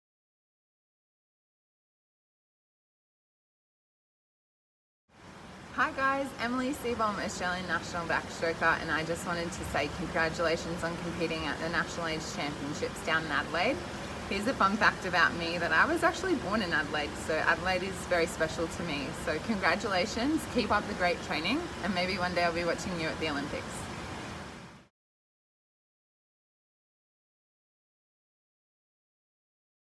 Emily Seebohm 2019 Australian National Age Message
Emily Seebohm congratulating you for competing at the 2019 Australian National Age Championships!

Emily Seebohm is an Australian competitive swimmer who has represented her country at the 2008 Summer Olympics, 2012 Summer Olympics and the 2016 Summer Olympics
Favourite Event - 100m Backstroke